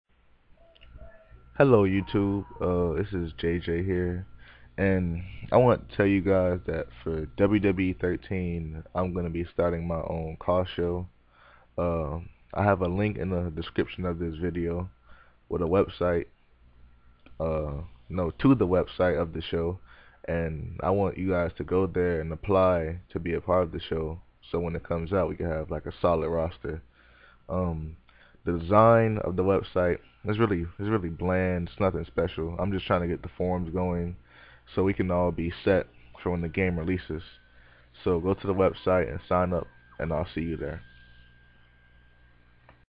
Im going to be making my own caw show for wwe 13.
Apply for an application and also read some of the other forums while your there!!